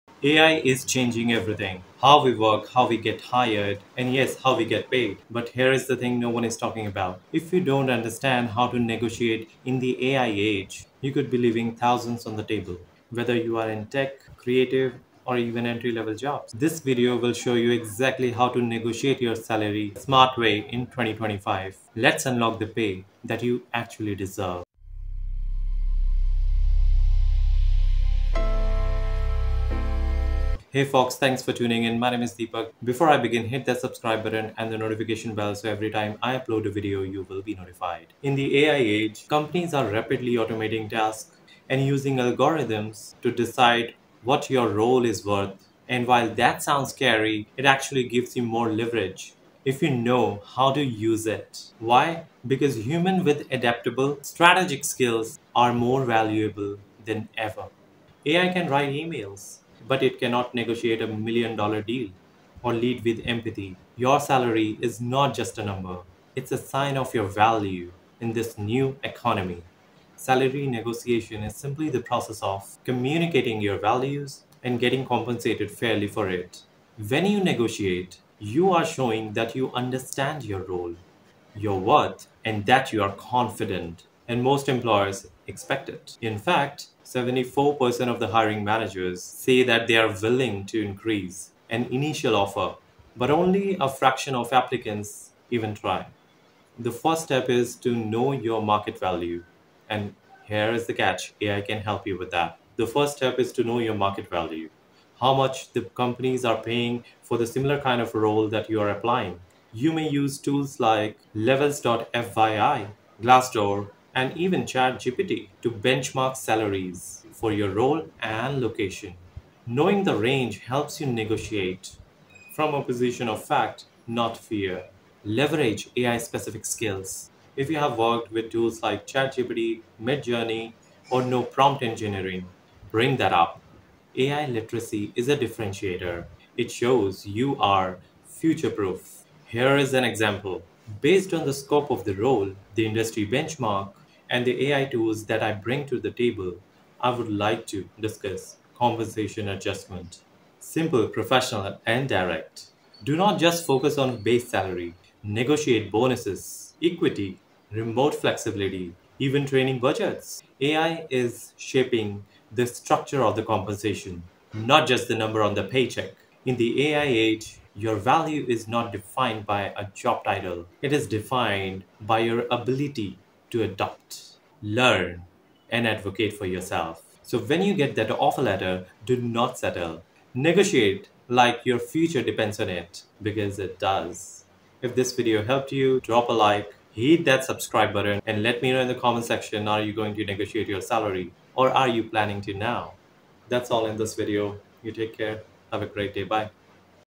Negotiate Your Salary in the AI Era 💼 | Boost Your Pay Using AI Skills!
Master Salary Negotiation with AI Expertise (Big Tech Secrets)

Are you getting paid what you're worth in the AI age? 💰
In this video, we dive deep into how AI and tech professionals can leverage their AI skills to negotiate higher salaries, stand out in a competitive job market, and secure the compensation you deserve in 2025 and beyond.

Whether you're a software engineer, data scientist, product manager, or transitioning into AI, these tips will help you:

✅ Showcase your AI expertise effectively
✅ Communicate your market value with confidence
✅ Use AI tools to research salary benchmarks and trends
✅ Tackle recruiter and HR conversations strategically
✅ Avoid common salary negotiation mistakes in tech